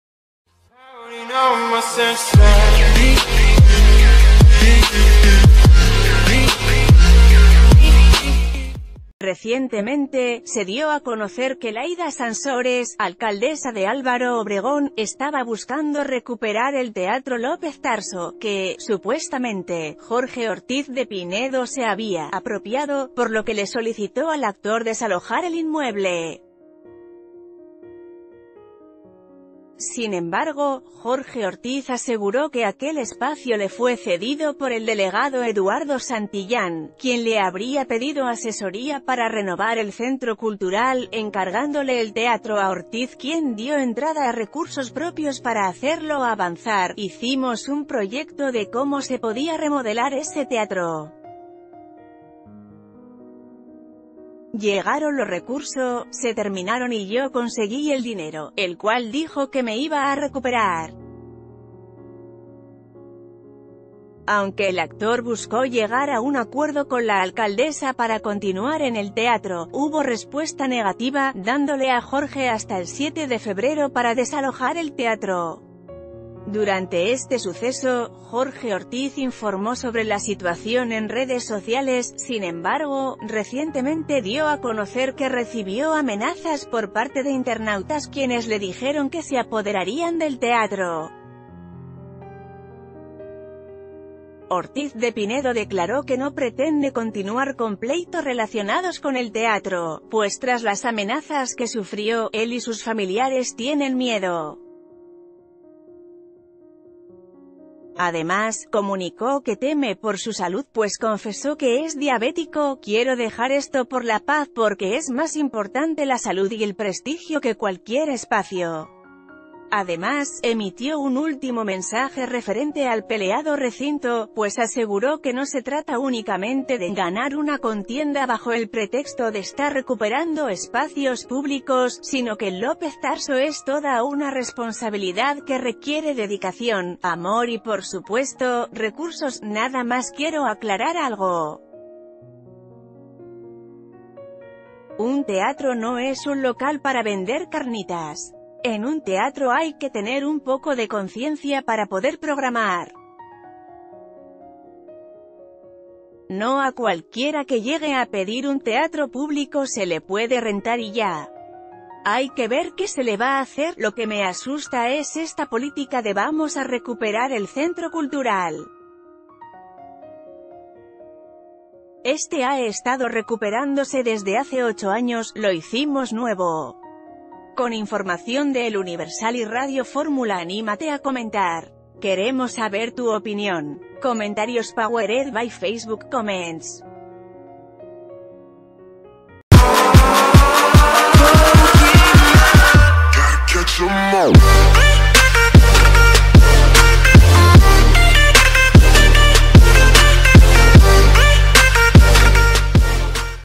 Jorge Ortiz de Pinedo confiesa que teme por su vida tras bronca laboral
Recientemente, se dio a conocer que Layda Sansores, alcaldesa de Álvaro Obregón, estaba buscando recuperar el Teatro López Tarso, que, supuestamente, Jorge Ortiz de Pinedo se había ‘apropiado’, por lo que le solicitó al actor desalojar el inmueble. Sin embargo, Jorge Ortiz aseguró que aquel espacio ...